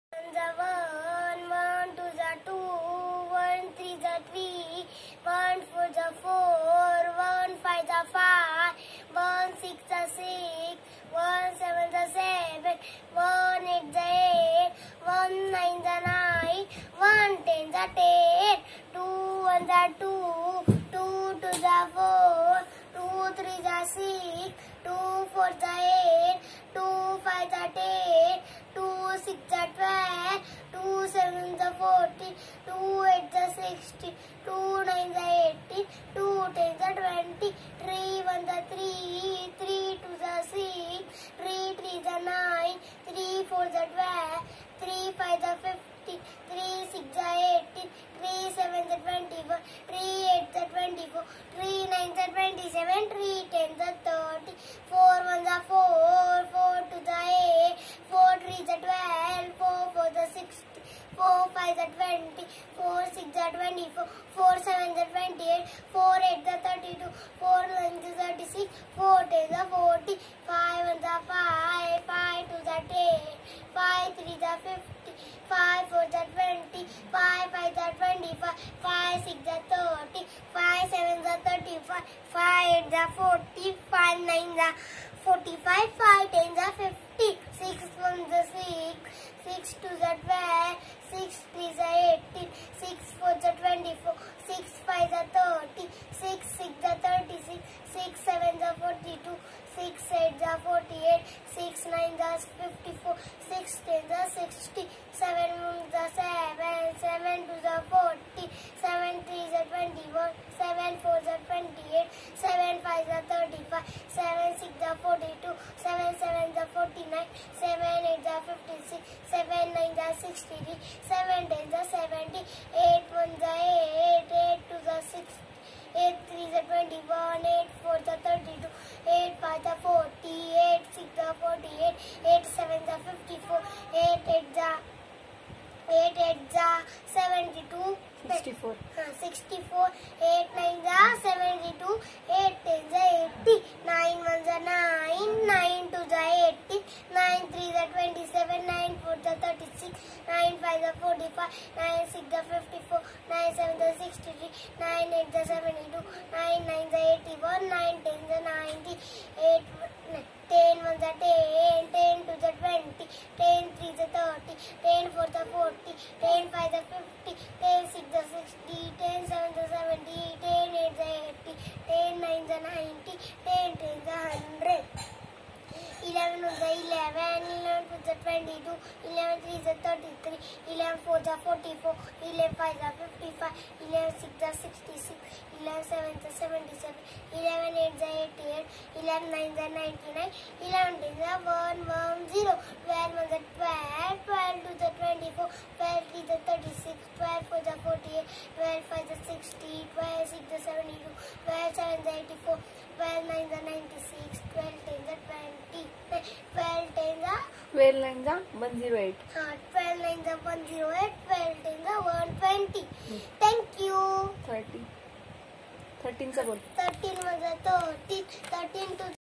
How many Tables Rudrani learn???? pls see here
नक्की पहा... Rudrani ने किती पाढे पाठ केले आहेत??? न चुकता बोलत आहे...